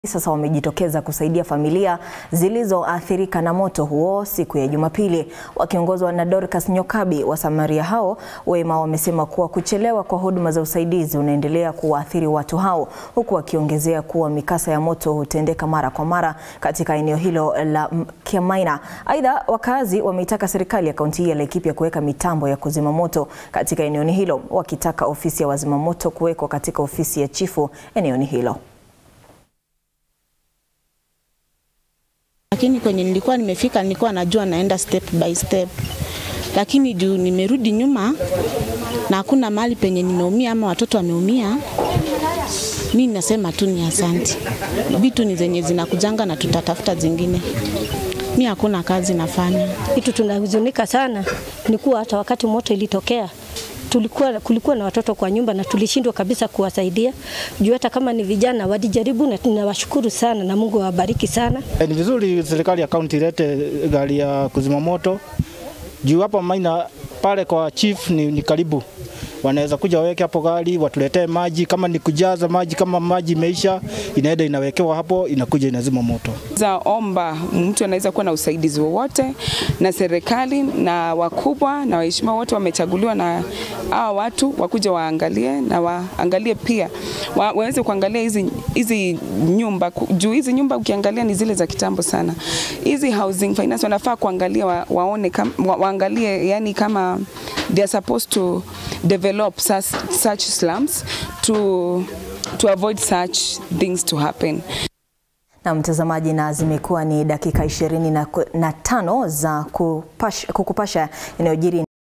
Wasamaria wema wamejitokeza kusaidia familia zilizoathirika Laikipia
Mkasa wa moto Laikipia uliwateketeza watoto wawili.

Wasamaria wema wamejitokeza kusaidia familia zilizoathirika.

Hii sio mara ya kwanza mkasa wa moto kutokea.

Wakaazi wametaka mikakati kuwekwa ili kukabili majanga.